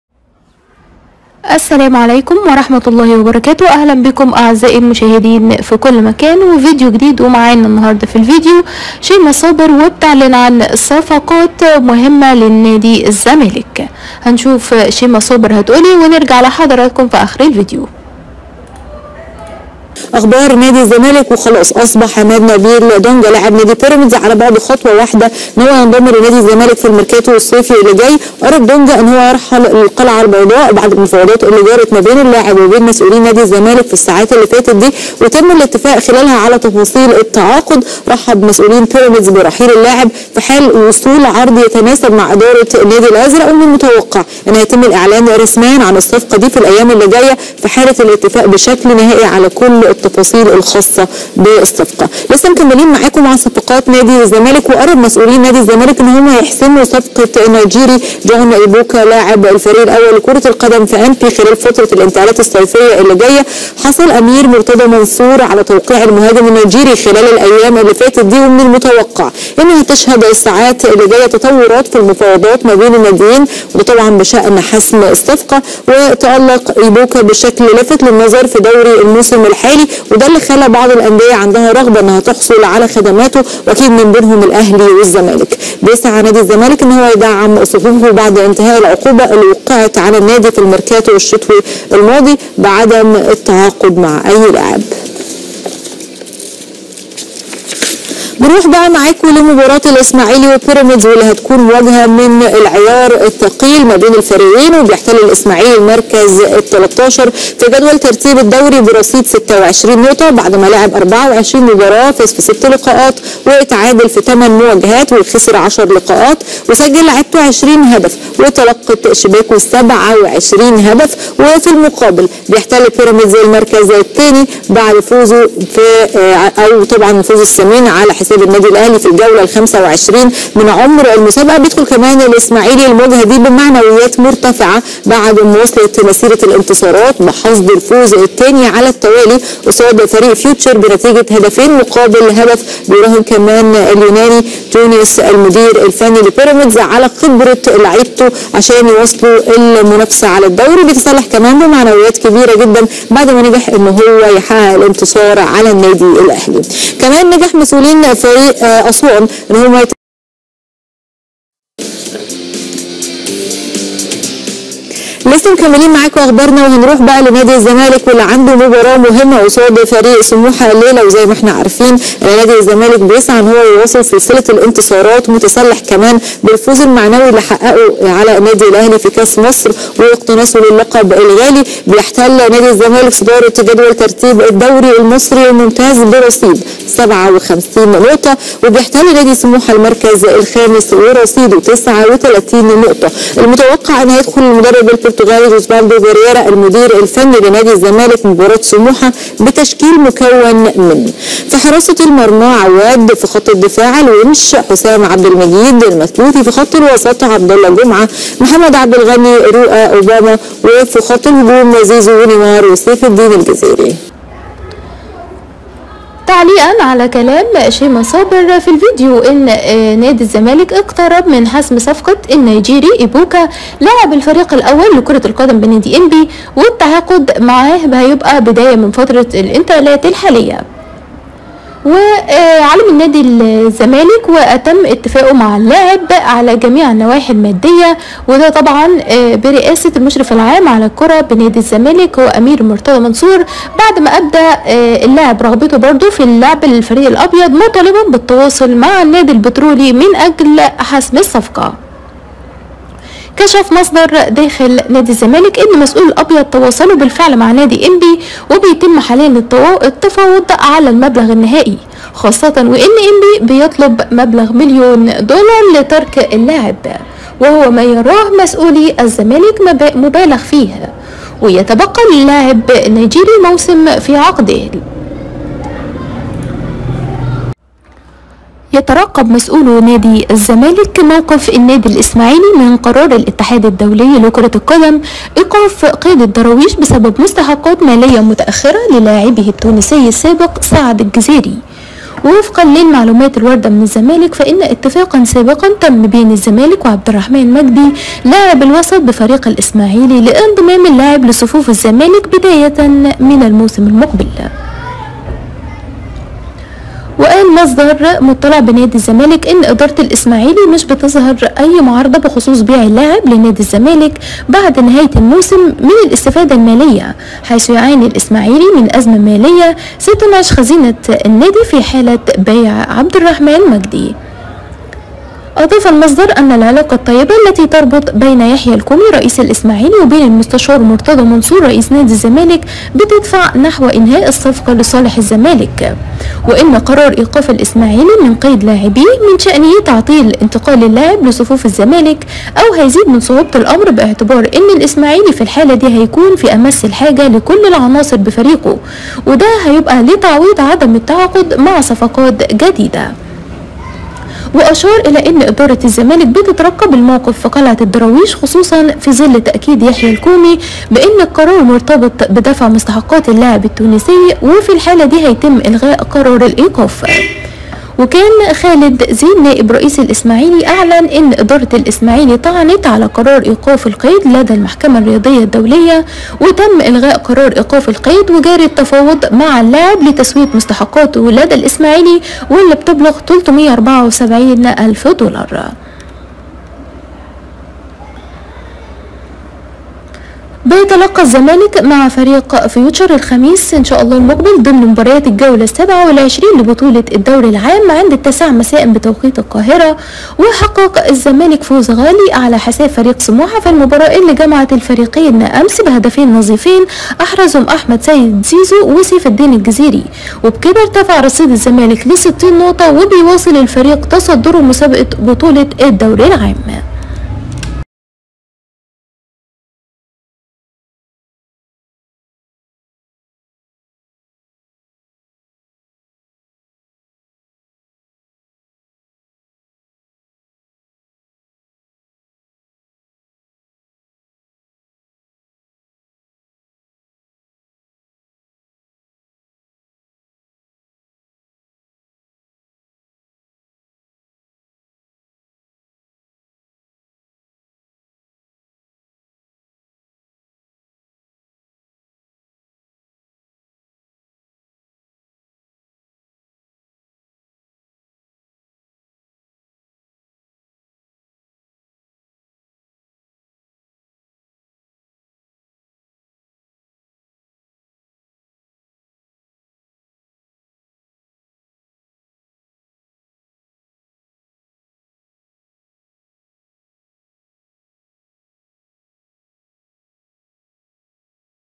شيما صابر تزف اخبار مفرحة بأمر من مرتضي منصور وحسم صفقتين سوبر من العيار الثقيل هيغيروا مصير الدوري
الرجاء الإشتراك في القناة وتفعيل علامة الجرس ليصلكم المزيد من الفيديوهات